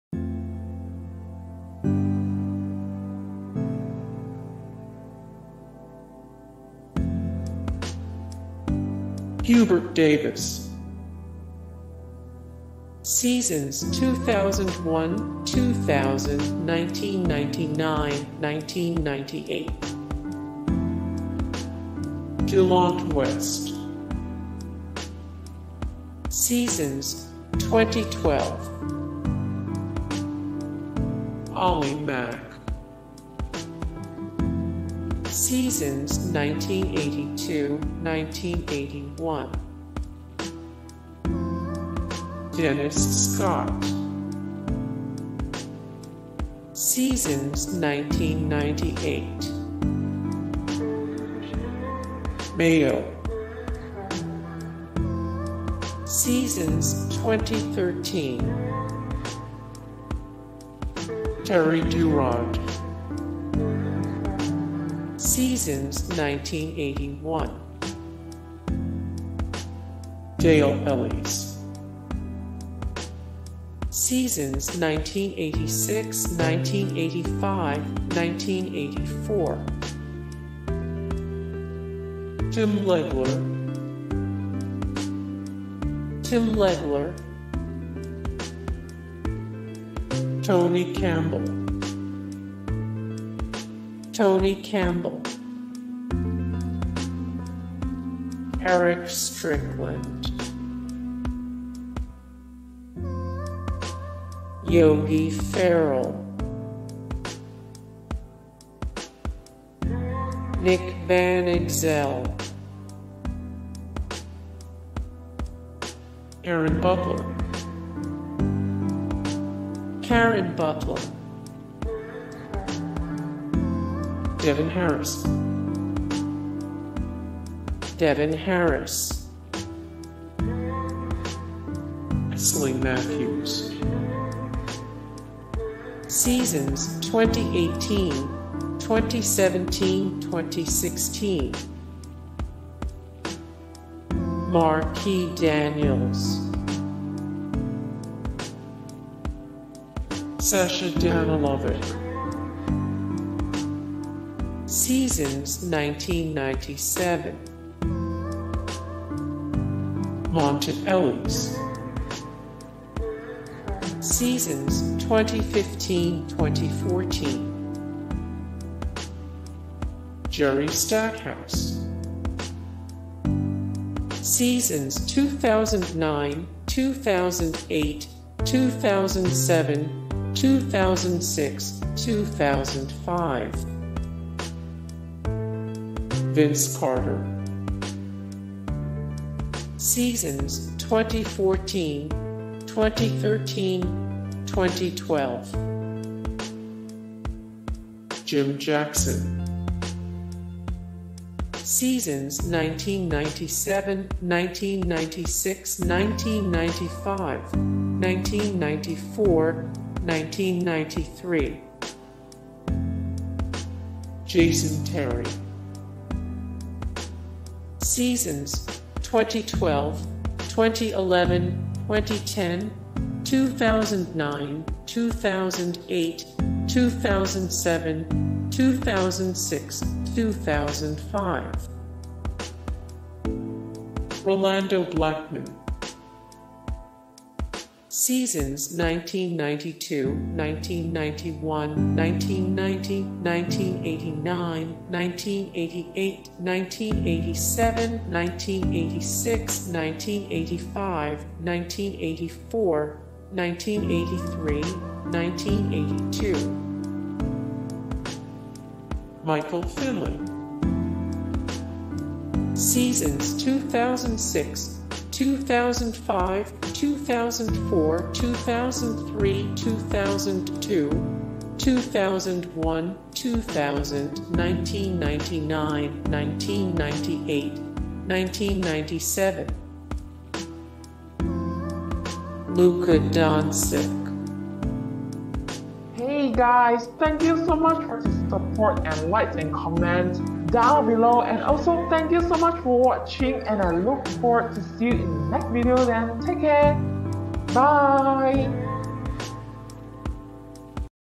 The Best Dallas Mavericks Shooting Guards Of All Time 💟
Big fan of Dallas basketball? Then check out this list of Dallas Mavericks shooting guards with photos and seasons played with the team.It includes almost every shooting guard from throughout Dallas Mavericks history, including current shooting guards and past ones. Featuring some of thegreatest NBA shooting guards of all time,the best MavericksSGs include Michael Finley, Rolando Blackman, Jason Terry, and Jim Jackson.In the 2017-18 NBA season, the current Dallas Mavericks starting SGs and backup SGs are Wesley Matthews, Seth Curry, and Gian Clavell.Who are the best shooting guards in Dallas Mavericks history? Here is every player who has ever held the position of Dallas Mavericksshooting guard.Photo: Michael Finley/Twitter...more
0:00 - Intro
0:00:08 - Hubert Davis
0:00:22 - Delonte West
0:00:32 - Ollie Mack
0:00:44 - Dennis Scott
0:00:55 - O. J. Mayo
0:01:05 - Terry Duerod
0:01:15 - Dale Ellis
0:01:28 - Tim Legler
0:01:37 - Tony Campbell
0:01:46 - Erick Strickland
0:01:53 - Yogi Ferrell
0:02:00 - Nick Van Exel
0:02:07 - Caron Butler
0:02:16 - Devin Harris
0:02:25 - Wesley Matthews
0:02:38 - Marquis Daniels
0:02:45 - Sasha Danilovic
0:02:56 - Monta Ellis
0:03:07 - Jerry Stackhouse
0:03:24 - Vince Carter
0:03:37 - Jim Jackson
0:03:54 - Jason Terry
0:04:15 - Rolando Blackman
0:04:41 - Michael Finley
0:05:05 - Luka Doni